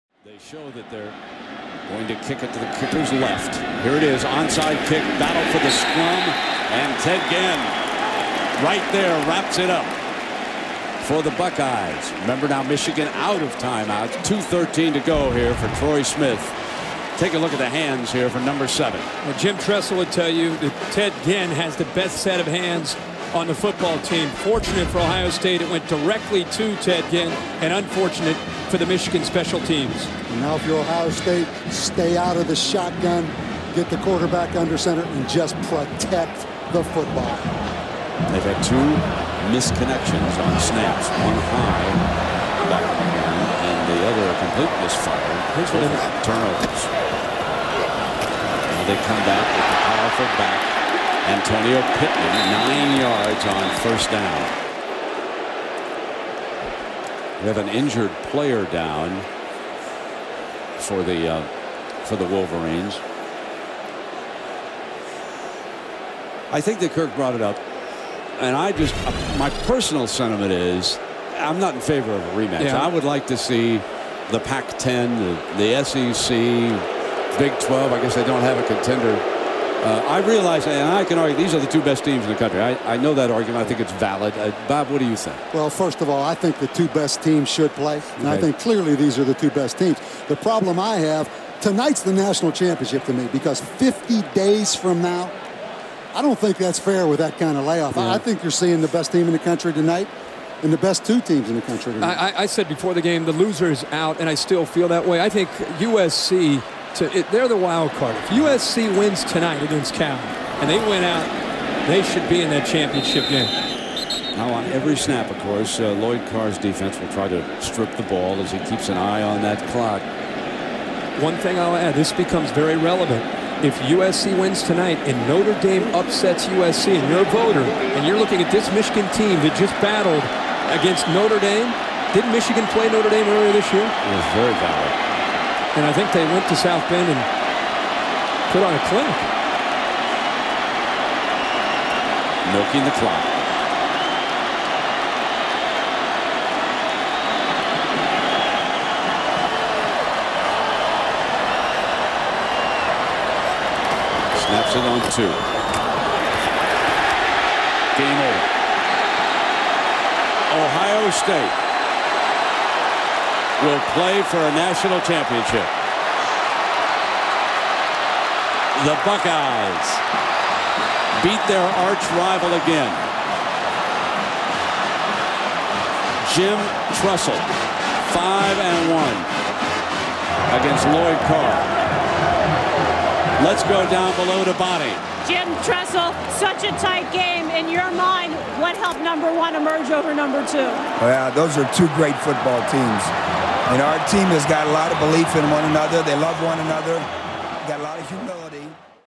Ohio State vs ttun
Another win over the skunk weasels. The soundtrack over the photos is from the 2006 game, just for fun